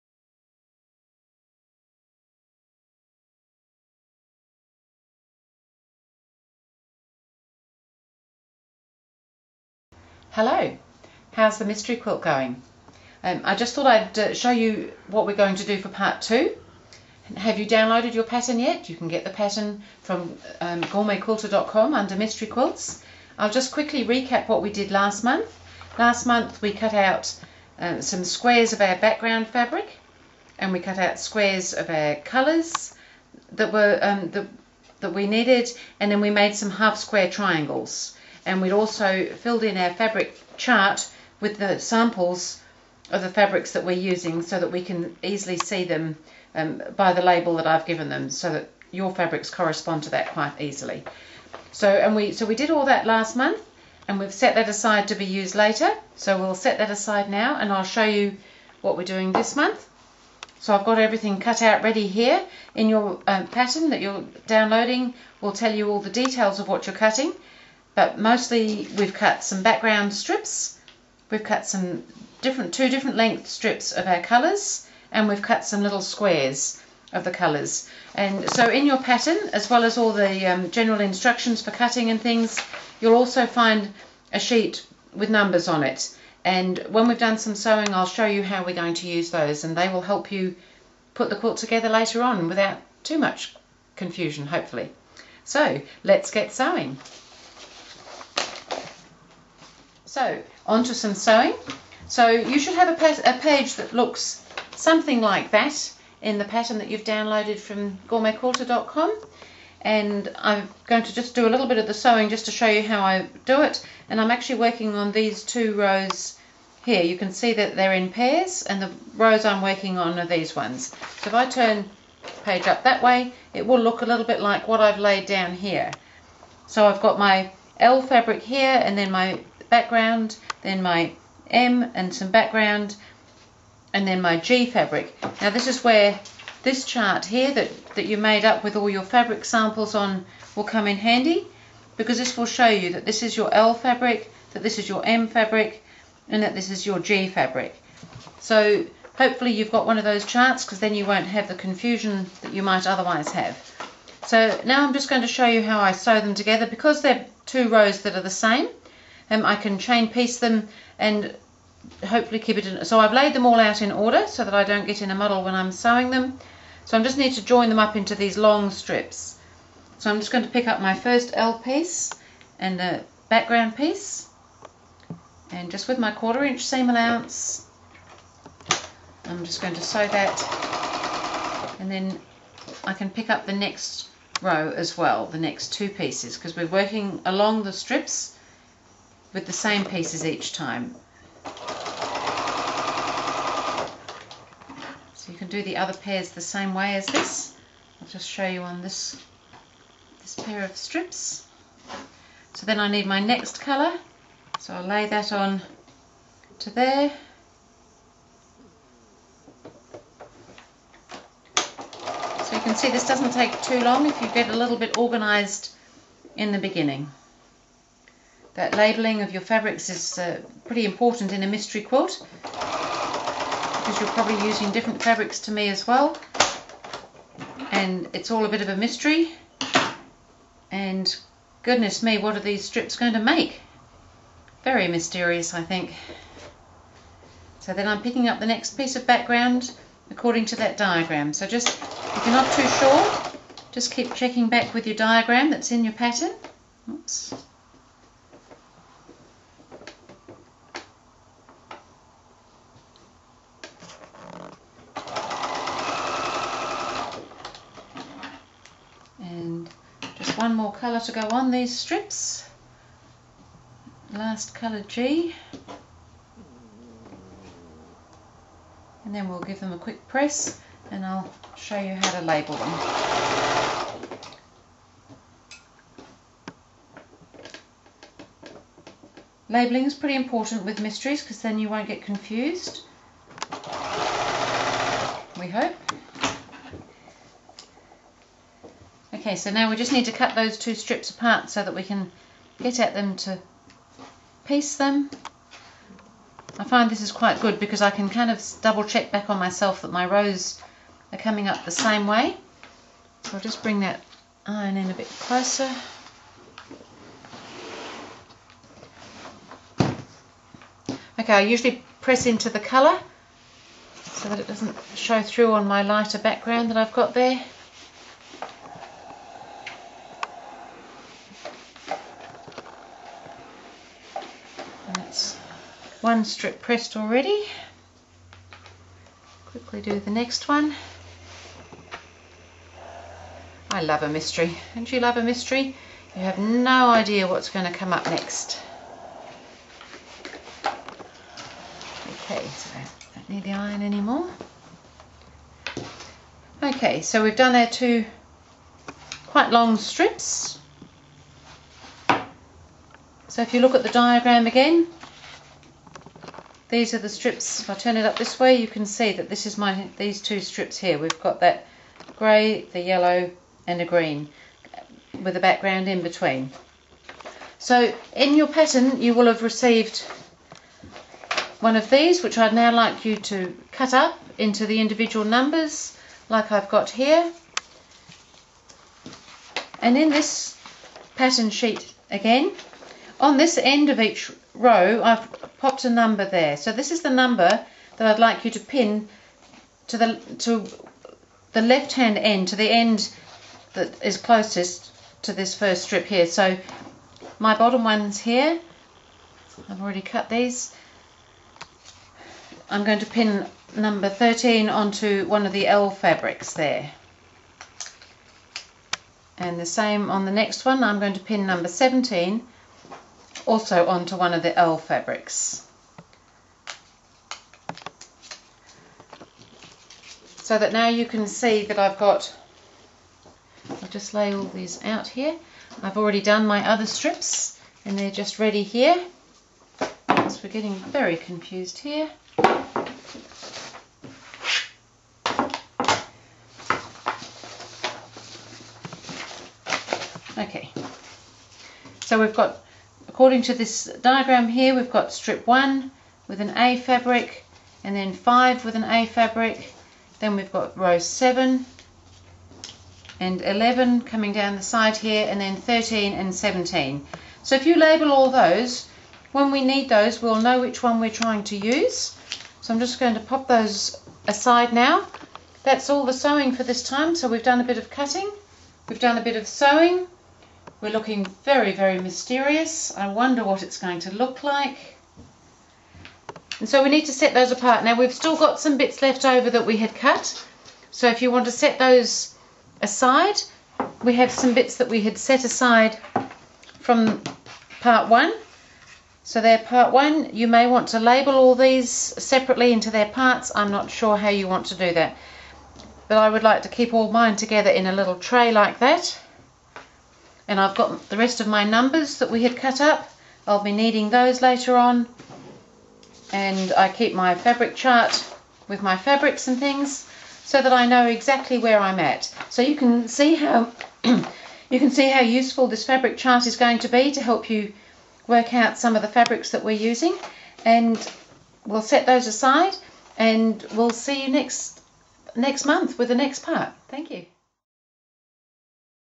Mystery Quilt Part 2 -That Chain Reaction Quilt - Free on-line mystery
oh, I just love a mystery!
This video offers a little encouragement and information about the free on-line mystery quilt pattern, available This free downloadable pattern will run over 6 months, this is Part 2,